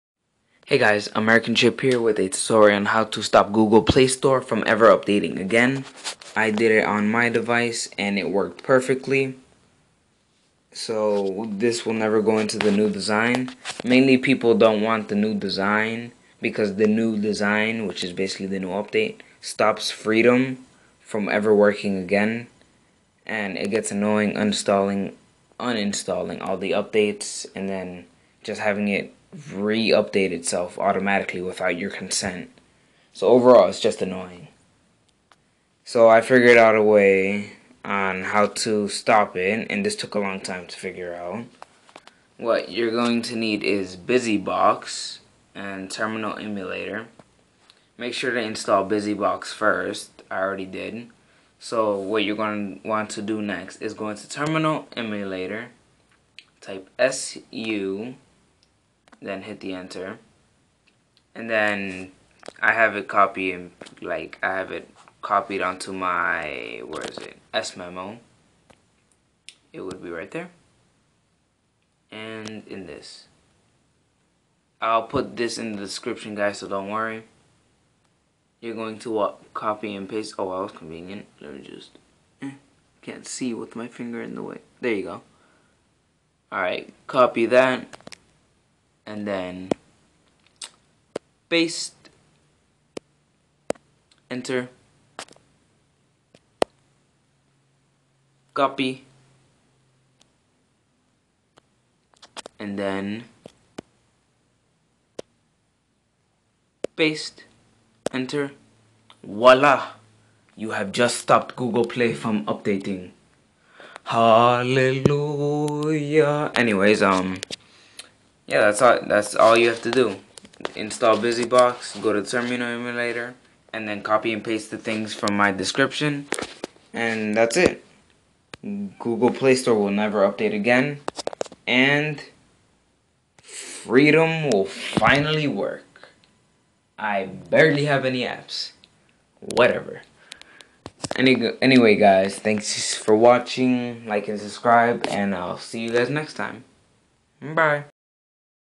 How to stop Google Play Store from auto updating
This does require root access and this method does work.
copy and paste into Terminal
1. su
2. touch /data/app/com.android.vending-1.apk
3. chattr +i /data/app/com.android.vending-1.apk